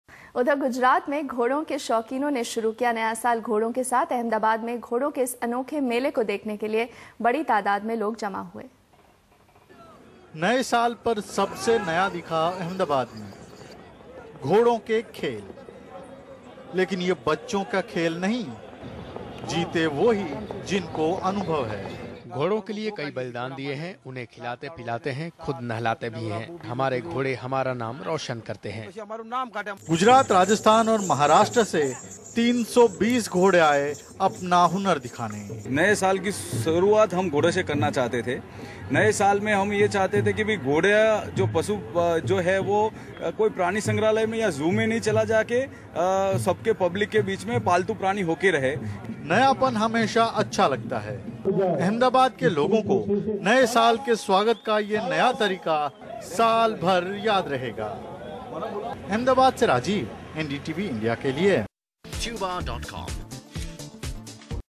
घोड़ों का मेला
गुजरात में घोड़ों के शौकीनों ने शुरू किया नया साल। घोड़ों के मेले को देखने के लिए बड़ी तादाद में लोग जमा हुए।